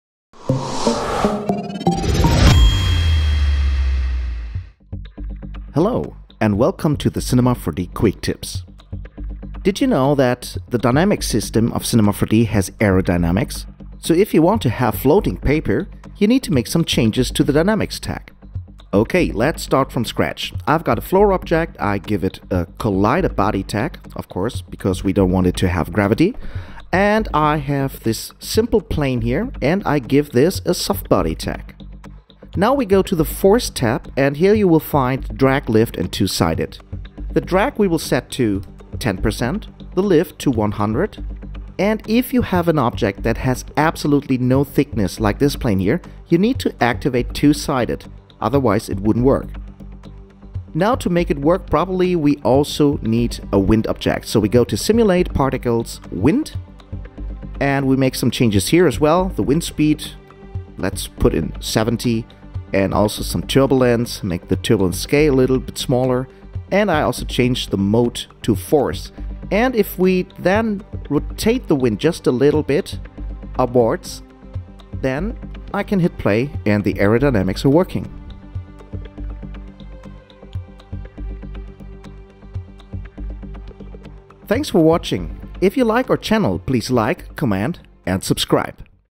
Tip - 251: Make paper float in Cinema 4D
In this Cinema 4D Quick Tip Glenn Frey shows you how easy it is to make paper float by activating Aerodynamics in Cinema 4D.